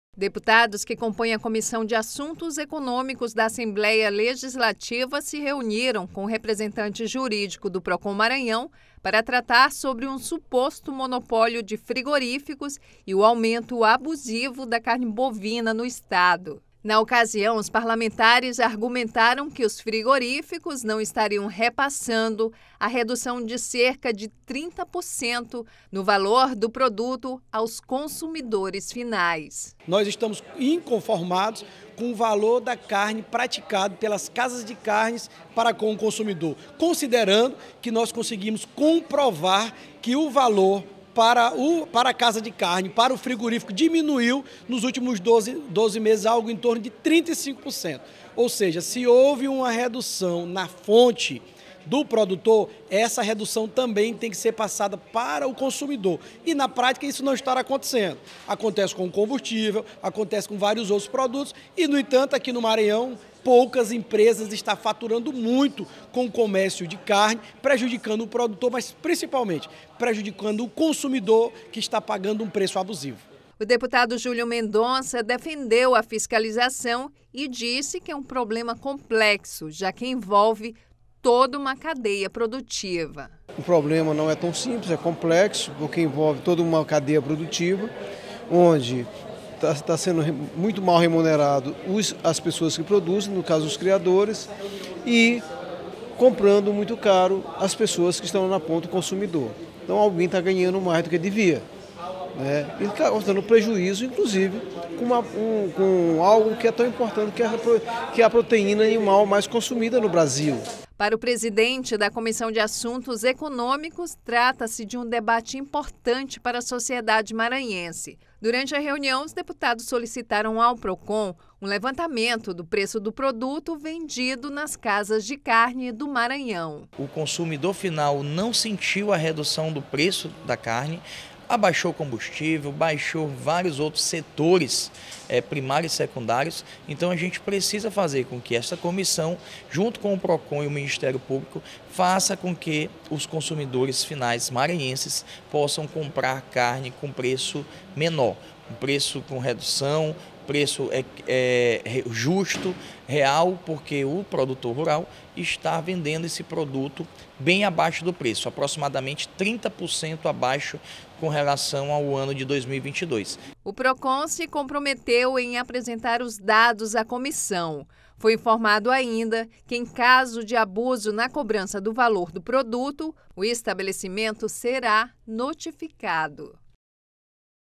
Comissão de Assuntos Econômicos recebe PROCON-MA para discutir 'preço abusivo da carne bovina'
A Comissão de Assuntos Econômicos da Assembleia Legislativa do Maranhão recebeu, na tarde desta quinta-feira (25), o assessor jurídico do PROCON/MA, Ricardo Cruz, para discutir suposto monopólio de frigoríficos e aumento abusivo do preço da carne bovina no estado, proteína animal mais consumida no Brasil.

A reunião, coordenada pelo presidente da comissão, deputado Francisco Nagib (PSB), contou com a presença dos deputados Eric Costa (PSD), Fernando Braide (PSD), Júlio Mendonça (PCdoB) e Yglésio Moyses. Os parlamentares explanaram a situação ao assessor jurídico e o indagaram sobre o trabalho de fiscalização do órgão.